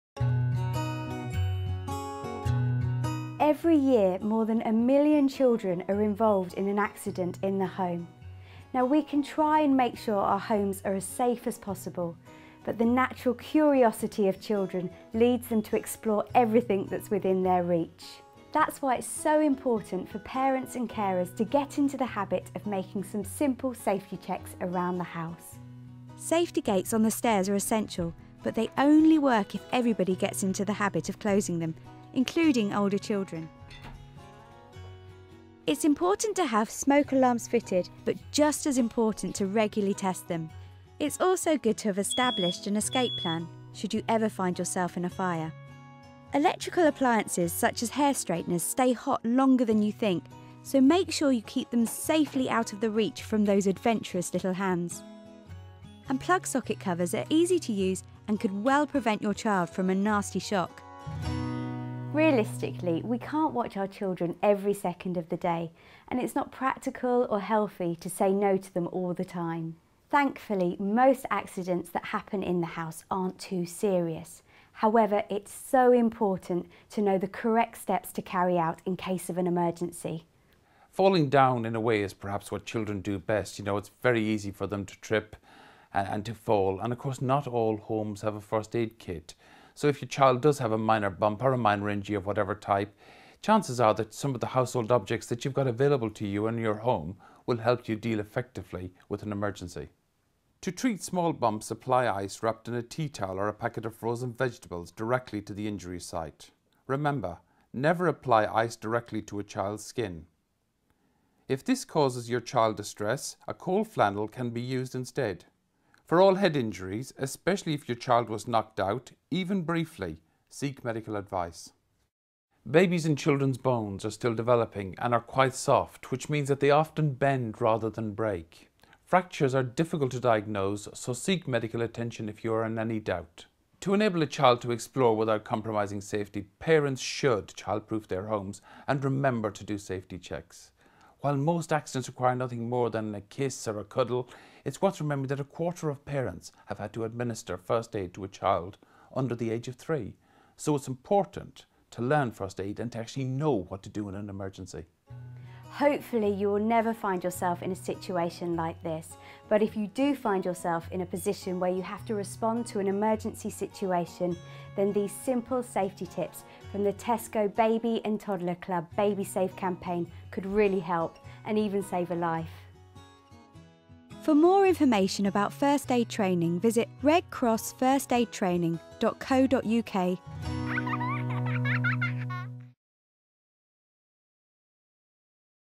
Tesco BabySafe video with Red Cross - Keeping kids safe in the home
This is the first in a series of Tesco Baby & Toddler Club Babysafe videos, and focuses on keeping children safe in the home.

It also shows how to deal with bumps to the head and falls.